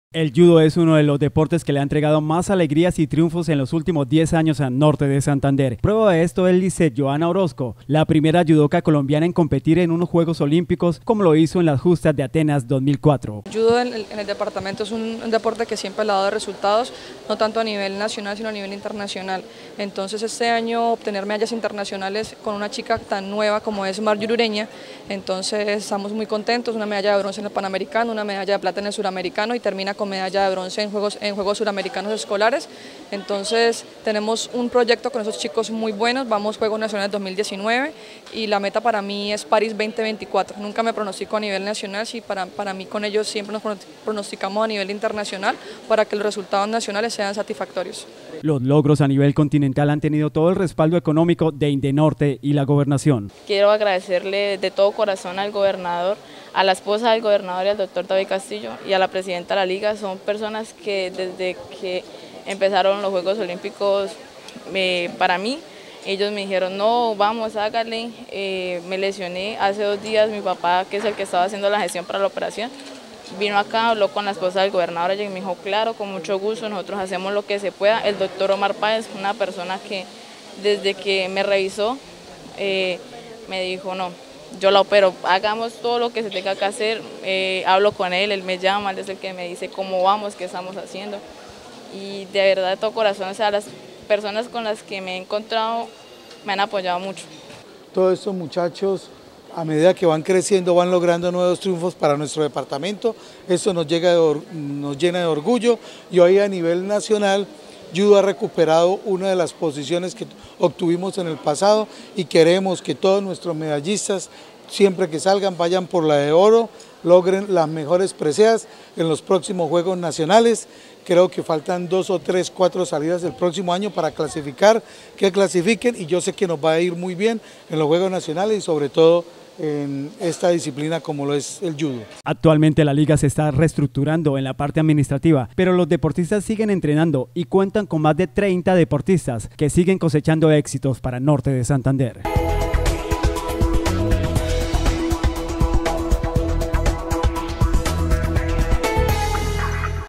Medallas INDENORTE
Ante los medios de comunicación la Liga Nortesantandereana de Judo, presidida por la excompetidora olímpica Johana Orozco, y el director de Indenorte, David Castillo presentaron  las medallas y trofeos logrados este año en las diferentes competencias en las que participaron con el apoyo de la Gobernación.